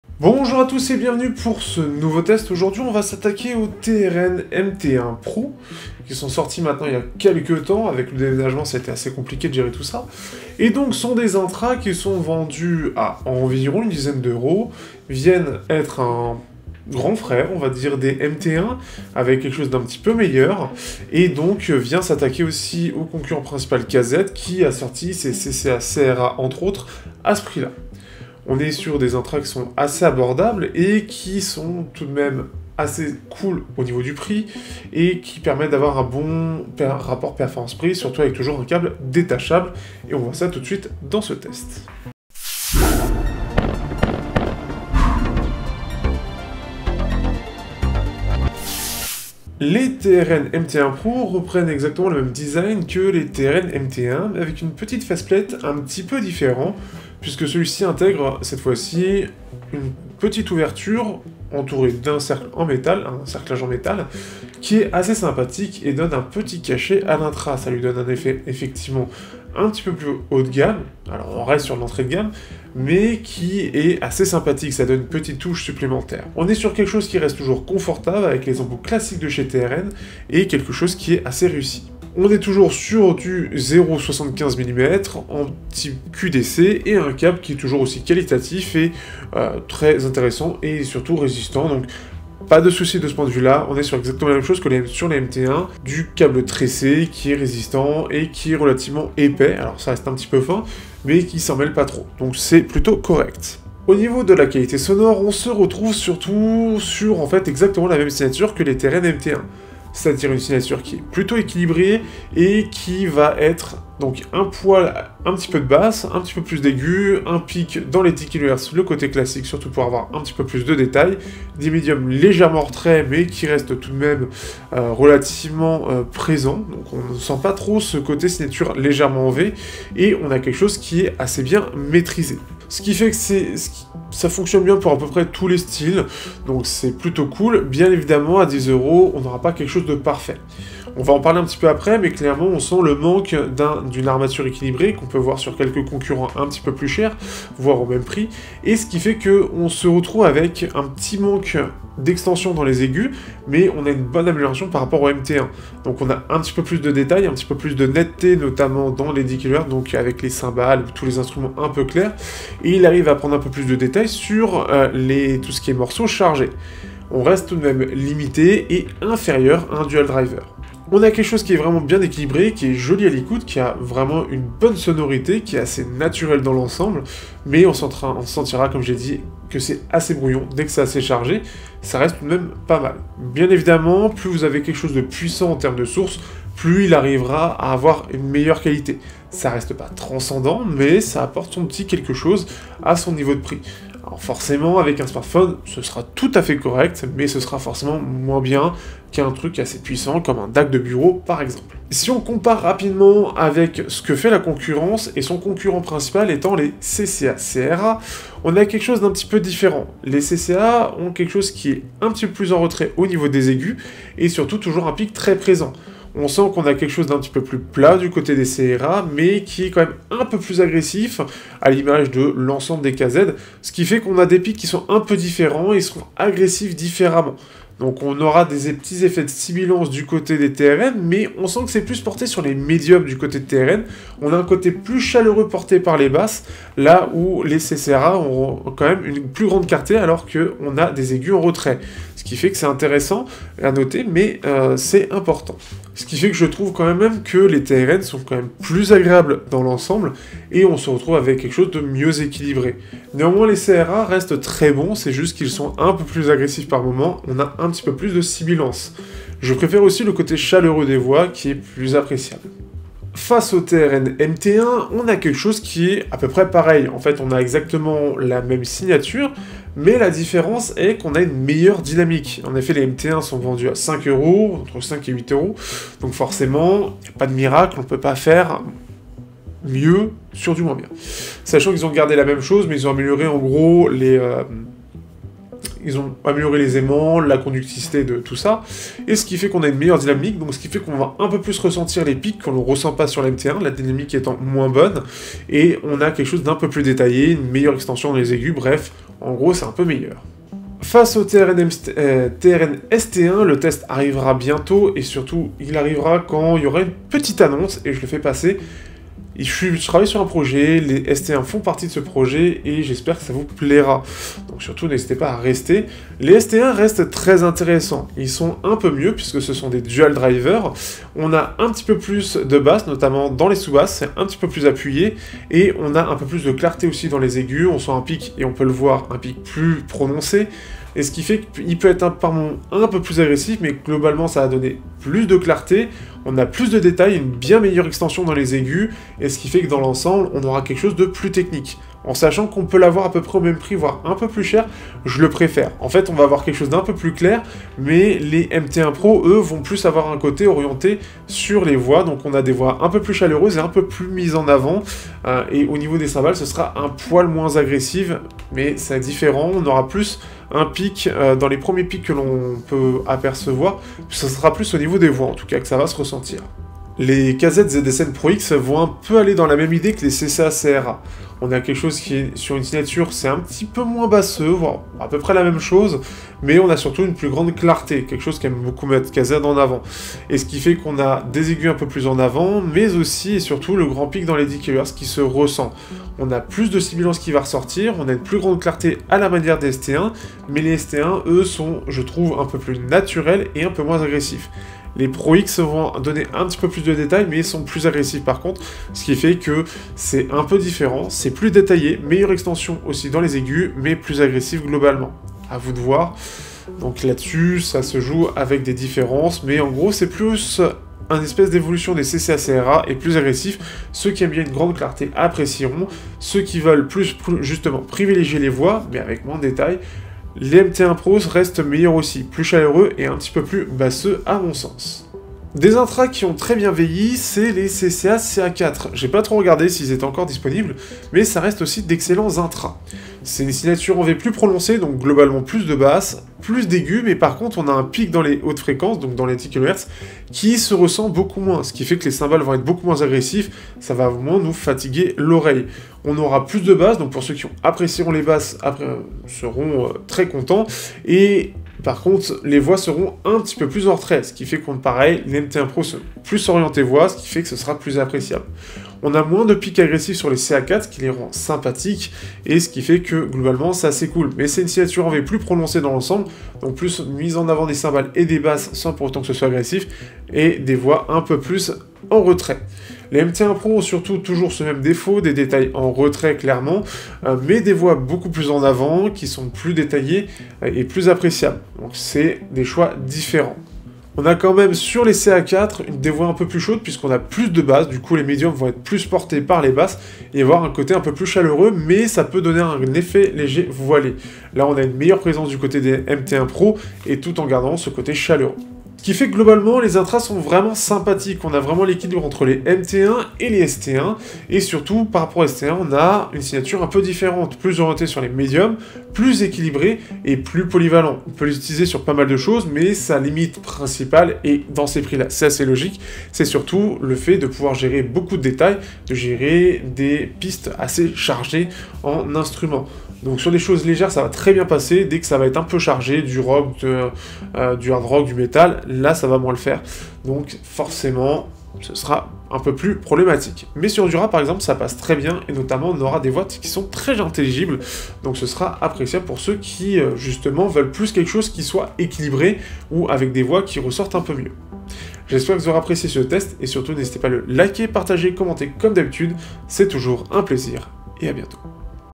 TRN MT1 PRO : Un son EQUILIBRE et CHALEUREUX pour PAS CHER
Dans les intras abordables il y a pas mal de choix mais souvent avec les mêmes réglages, avec les TRN MT1 et maintenant les TRN MT1 PRO un autre choix de qualité s'offre aux gens avec une signature un peu plus axé sur la voix et moins de gros pic agressifs

Merci aux tipeurs et uTipers

Mon Top entre 10 euros et 300 euros dans la Chifi

Mes Playlists musicales

Retour de commission sur Amazon ( permet de financer un nouvel APN